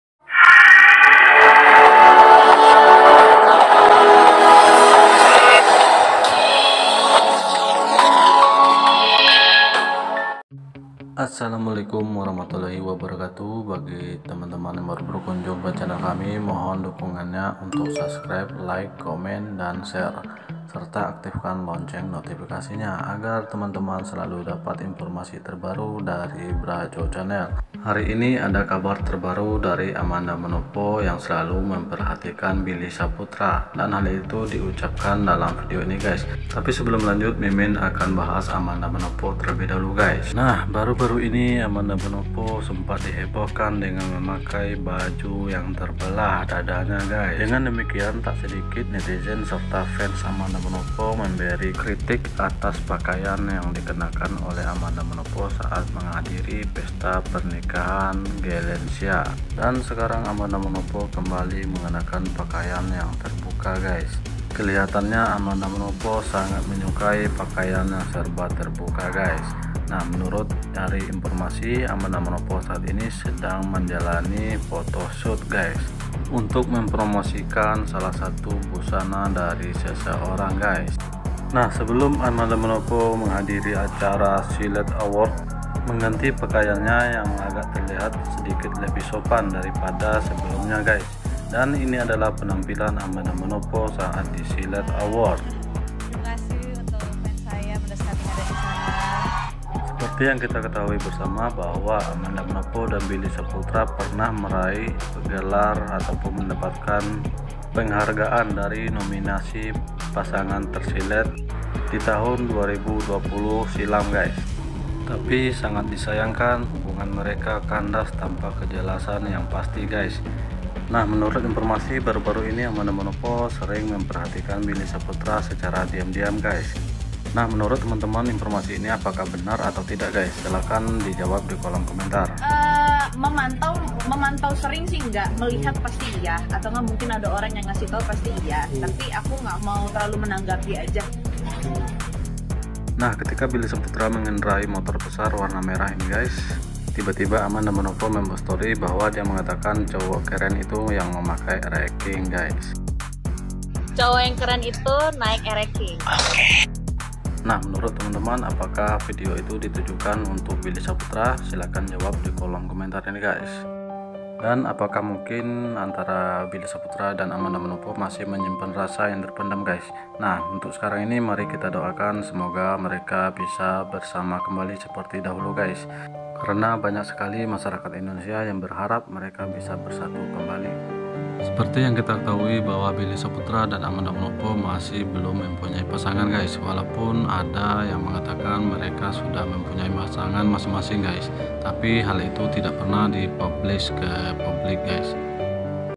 AMANDA MANOPO KASIH KODE BILLY SYAHPUTRA CLBK
TERBONGKAR diam diam AMANDA MANOPO PERHATIKAN BILLY SYAHPUTRA apakah akan clbk
hal itu terlihat di salah satu video yang ketika itu Billy Syahputra sedang mengendarai moge dan tiba tiba amanda membuat postingan bahwa ia mengatakan cowok keren itu pakai rx king..selain itu amanda manopo juga mengatakan bahwa dia sering memantau Billy Syahputra.